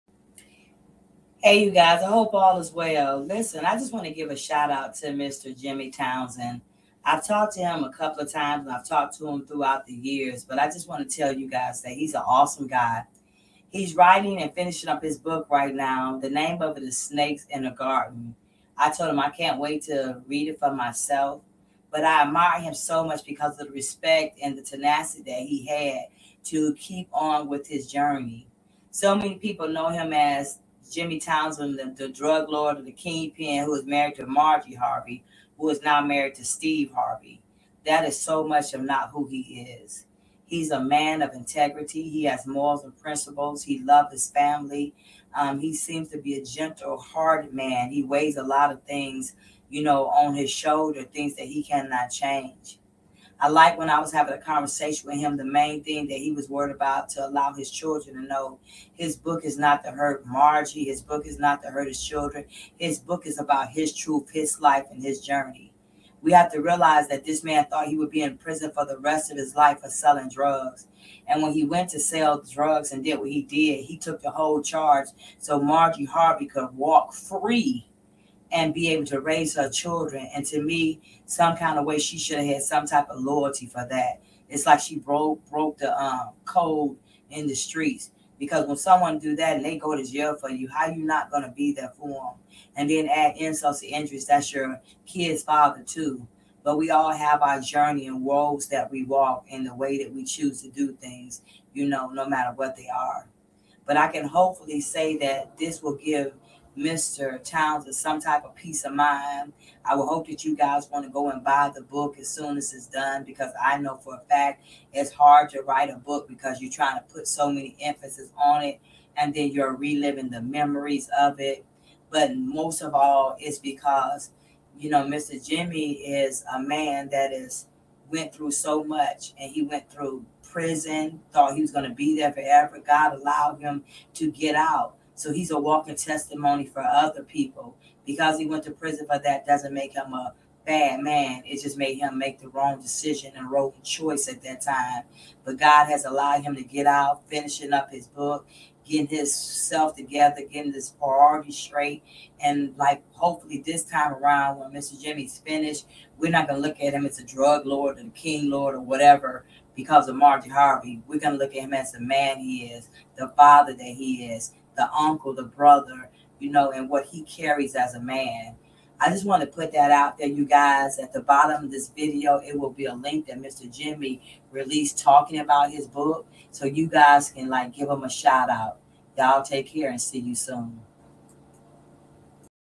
Jimmy Townsend tell all Book Snakes in the Garden, Before Steve Harvey it was Jimmy & Marjorie
Hello, my name is Jim L. Townsend and I'm telling my story in this memoir, a story of my journey filled with challenges and hardships. I'll take you through my experiences as I try to make sense of a world filled with child abuse, anger, fear, and addiction. You'll get a front row seat to see what it's like to face violence, drug dealing, my marriage to Marjorie Harvey (current wife of actor/comedian Steve Harvey) and greed, and how it can impact someone's life. My goal is to provide an intimate and raw look at what I went through, using vivid language to help you understand my journey through my eyes.
This memoir is for anyone who wants to delve into the complexities of the human experience and understand the strength of the human spirit. After reading it, I hope you'll feel inspired and empowered, and that you'll see a message of hope and perseverance. If you're looking for a real, honest, and powerful story, that is entertaining as well as informative, this memoir is for you. This is Part One of a Three Part Series. Enjoy!